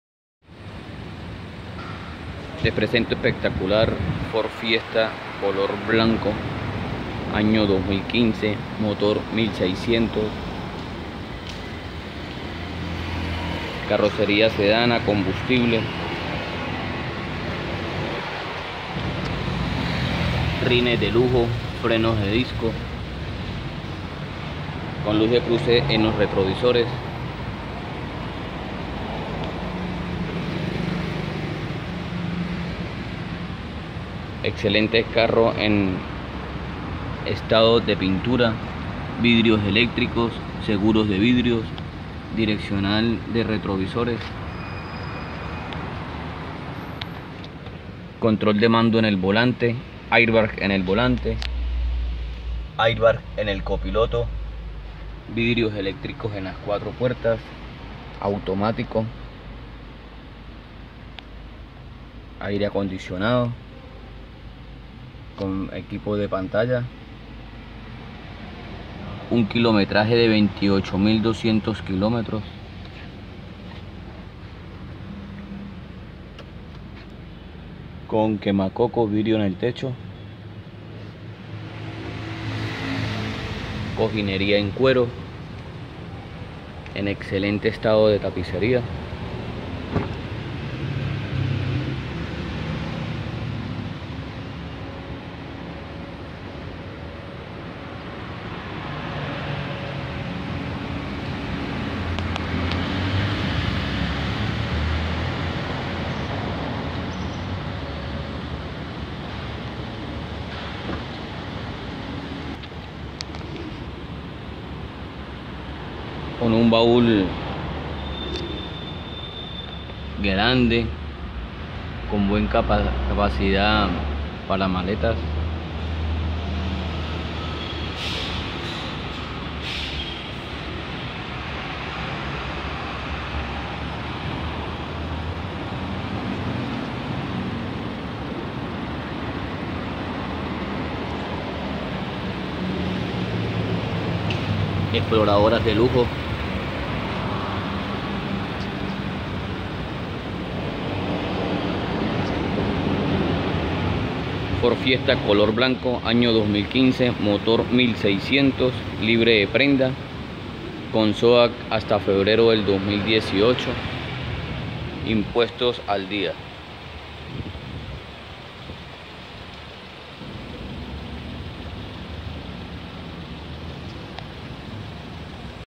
FORD FIESTA TITANIUM FULL EQUIPO 2015
MARCA: FORD
VERSION: FIESTA TITANIUM FULL EQUIPO
MODELO: 2015
MOTOR: 1.600
KILOMETRAJE: 25.000
TRANSMISION: AUTOMATICA (CAJA DUAL)
EXTERIOR: EXPLORADORAS, RINES DE LUJO, FRENO DE DISCO.
INTERIOR: CUADRO DE MANDO EN EL VOLANTE.
INFORMACION ADICIONAL: 27 INNOVACIONES, PERFECTO ESTADO DE CONSERVACION, PLACAS DE CUCUTA TERMINADA EN 8.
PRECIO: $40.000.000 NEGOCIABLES

 SE FINANCIA EL 100% DEL VALOR DE ESTE VEHICULO